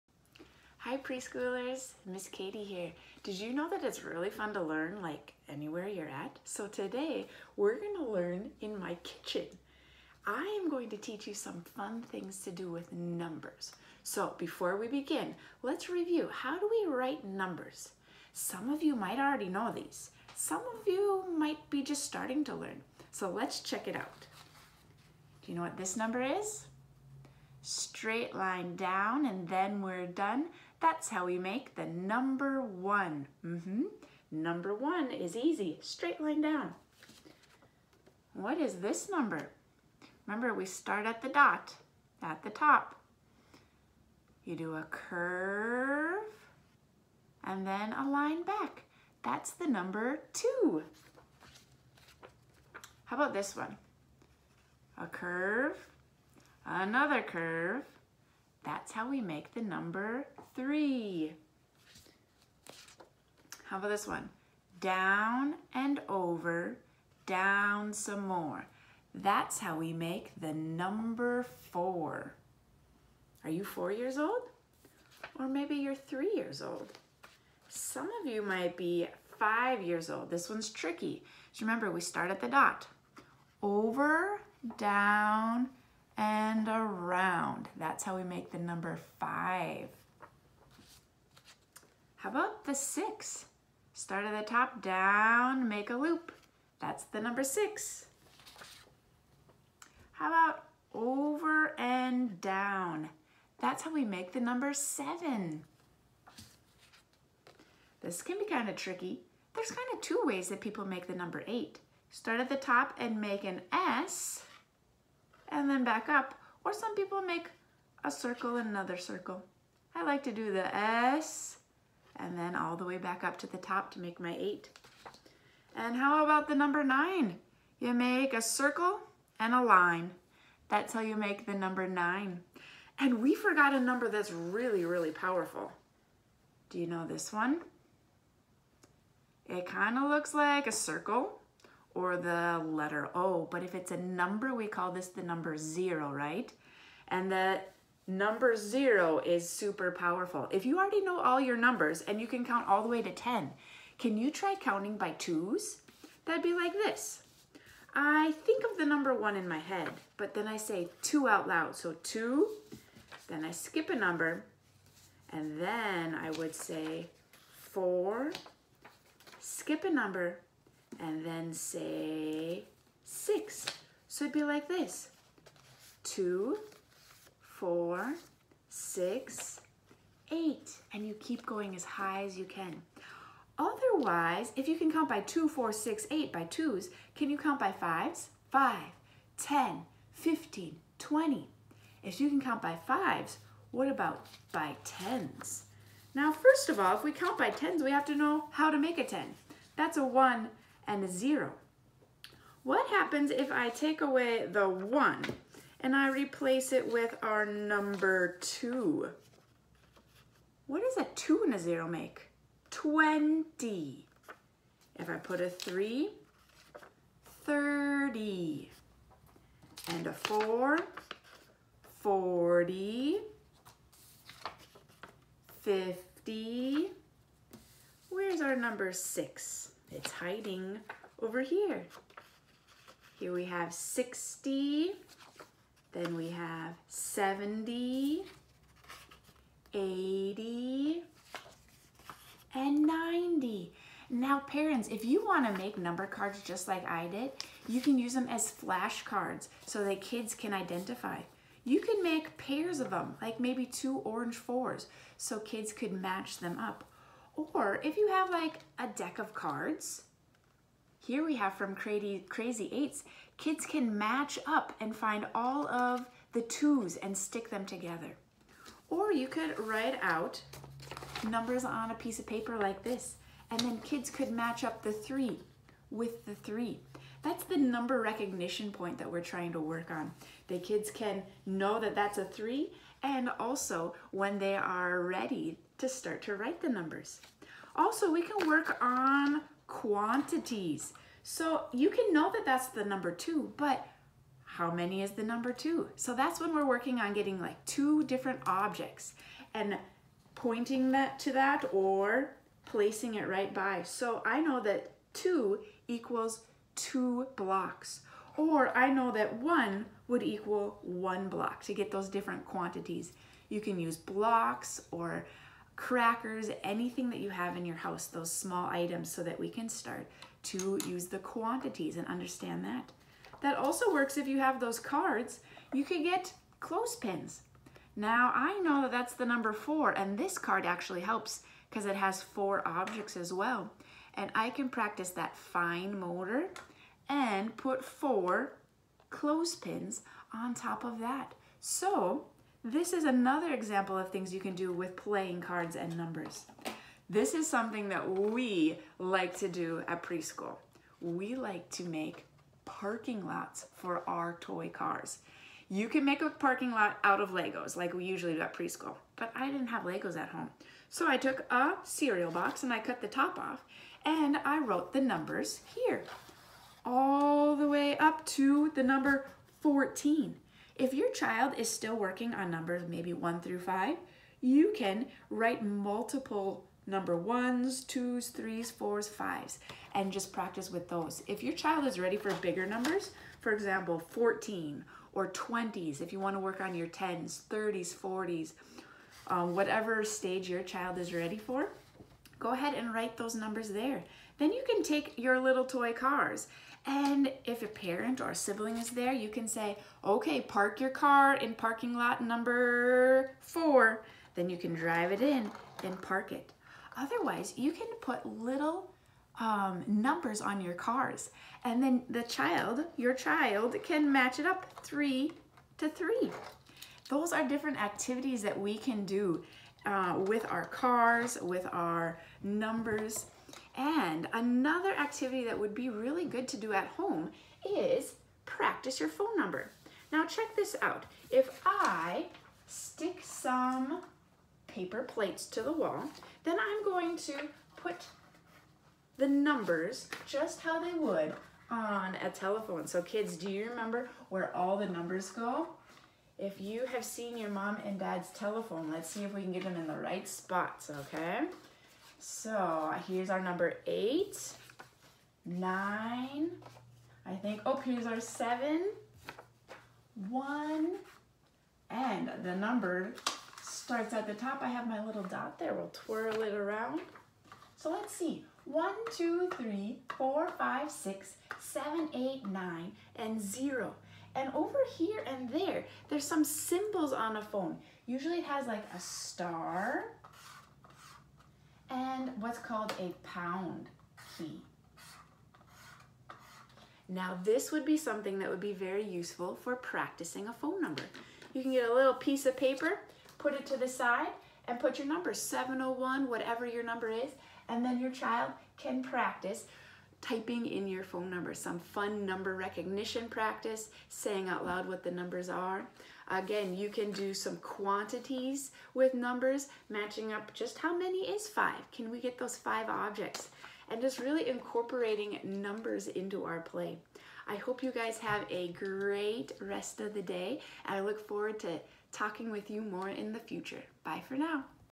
Math Games with Miss Katie
Write numbers, count by 2s, 5s, and 10s, number parking lot, match numbers with quantities, practice your phone number. Let's explore some math games you can play at home! Hosted by Miss Katie from St. John Christian Preschool in Fargo, North Dakota.